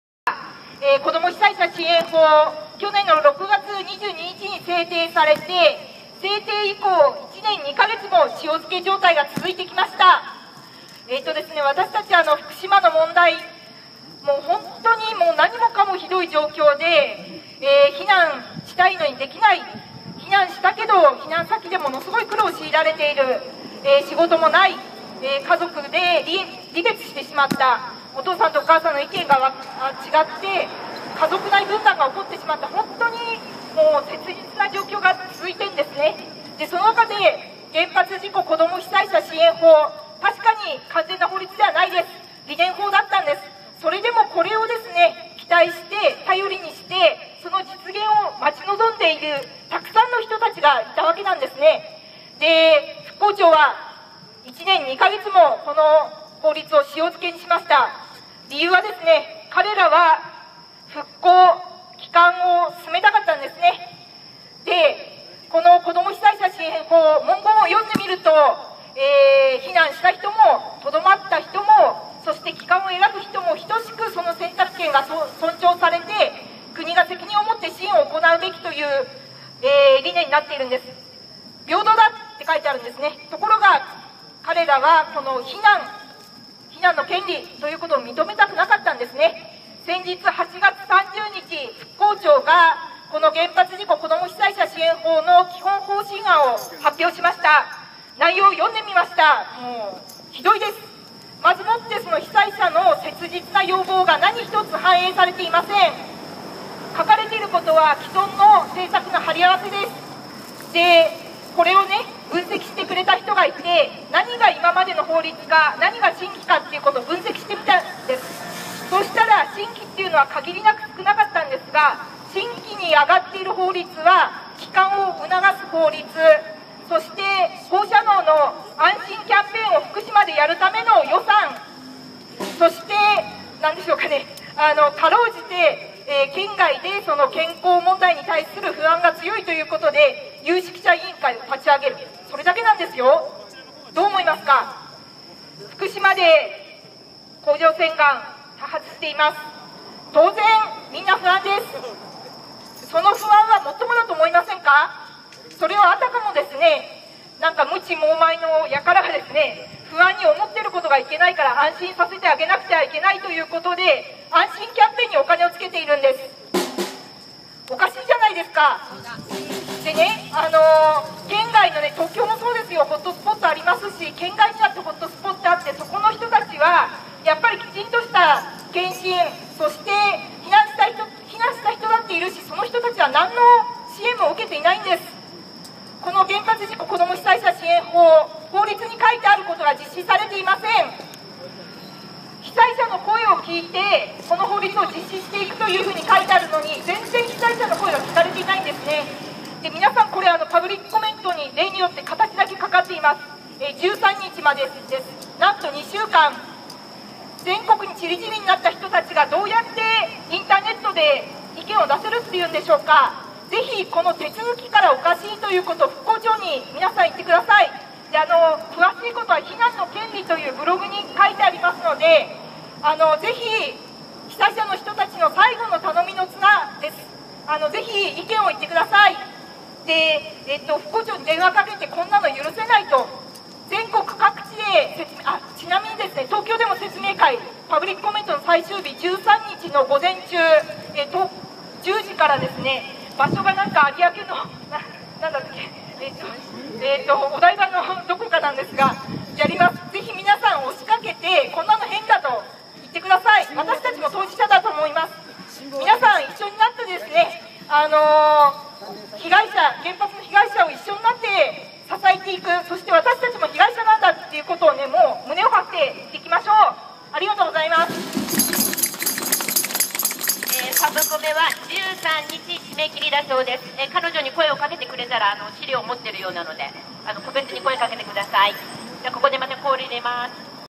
2013.09.06 官邸前抗議行動：満田夏花さん（原発事故子ども・被災者支援法）〈国会正門前〉
2013年9月6日 毎週金曜日恒例の「首相官邸前抗議行動」
主催：首都圏反原発連合

「国会前スピーチ・エリア」で、満田夏花さんが復興庁が出した〝原発事故子ども・被災者支援法〟の「基本方針案」の大きな問題点をスピーチ。

iPhone 4S で撮影。